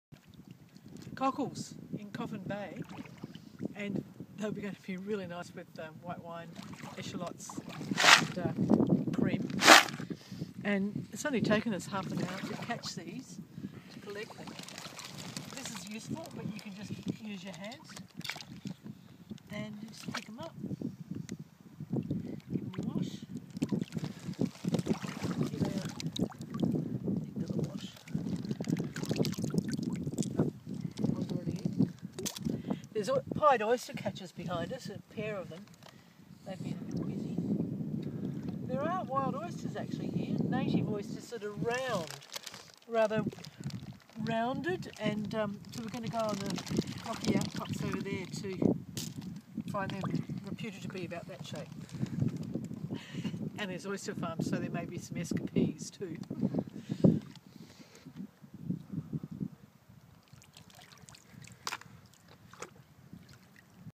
Wild Clam Harvest in Eyre Peninsula Australia
Lots of great and clean seafood to be harvested  in Coffin Bay.
No industry just a clean sea.